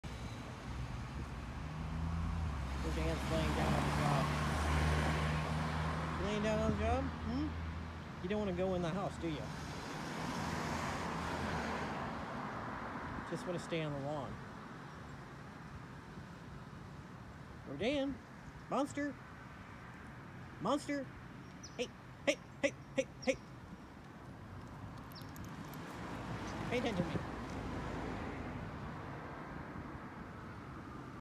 Schipperke Dog Rodan Enjoying The Lawn
My Schipperke doesn't want to go into the house. Loves the front lawn.